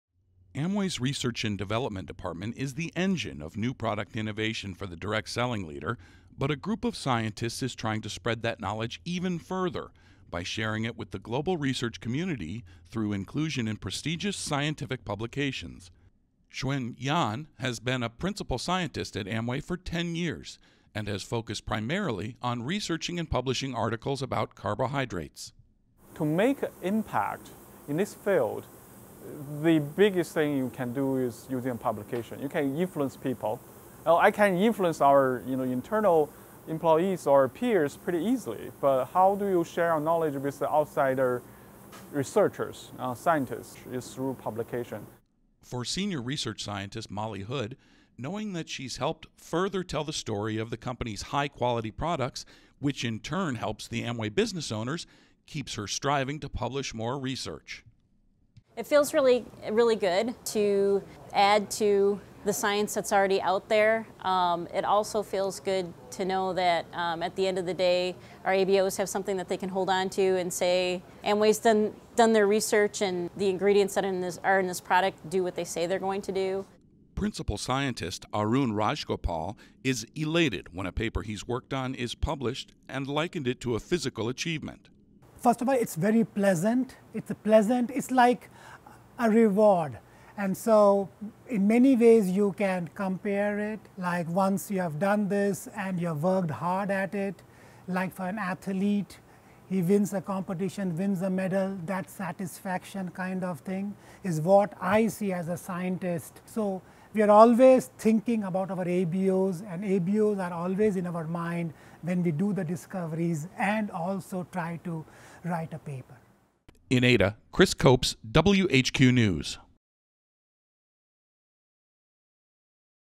Amway scientists participate in publications | WHQ News
Amway employs an army of scientists, engineers and technical professionals who work on the company’s next great products. While efforts behind some segments of research are kept secret for competitive purposes, others warrant a different approach: Sharing. WHQNews takes a look.